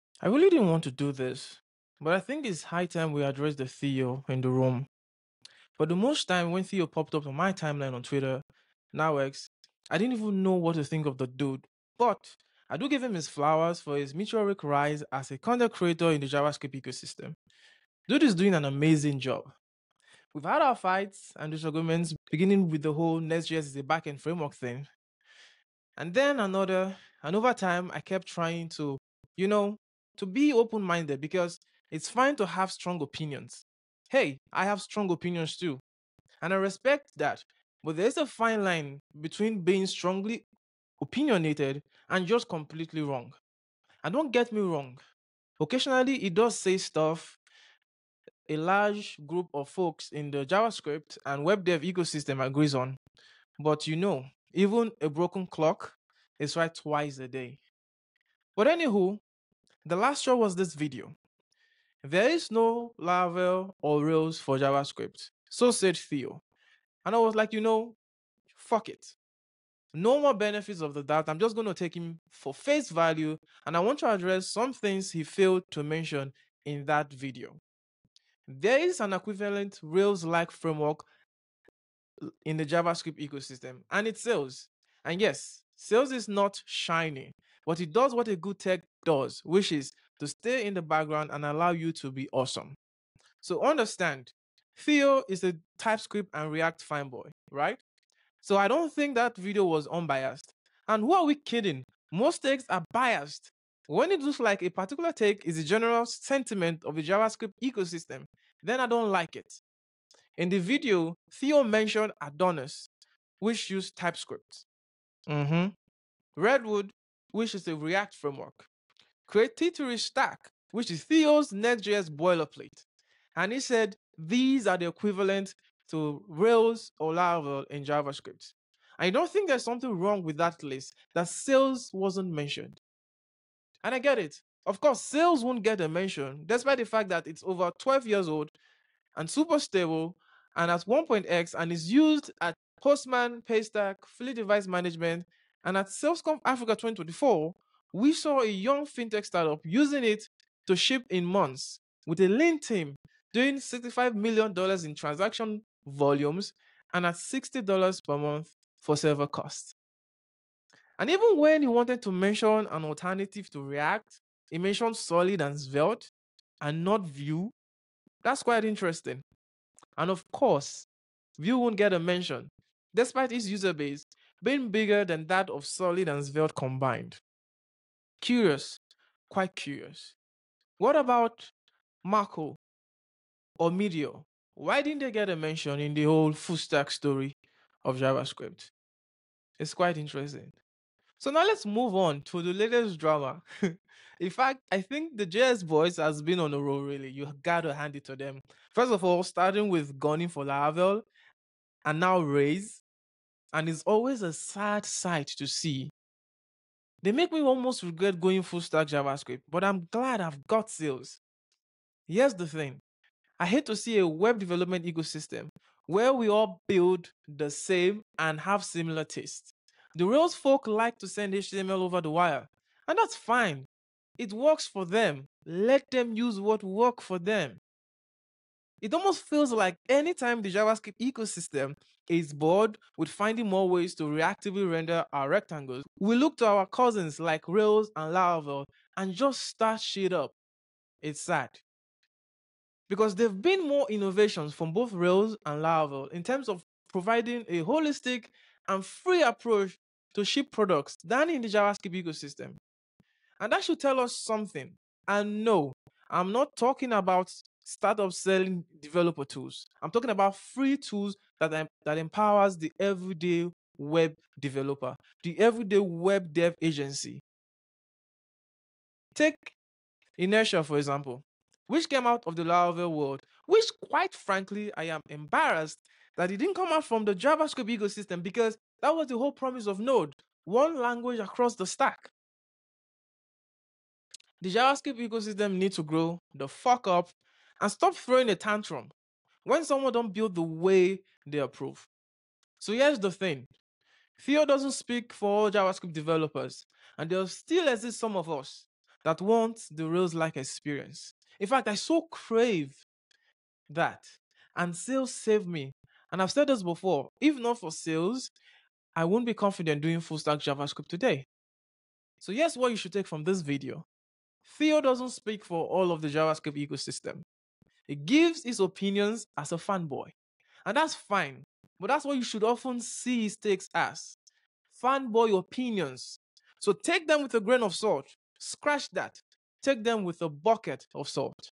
The Theo Problem
Theo doesn't speak for all of the JavaScript ecosystem.

LINKS 🔗
Sails

QUESTIONS...?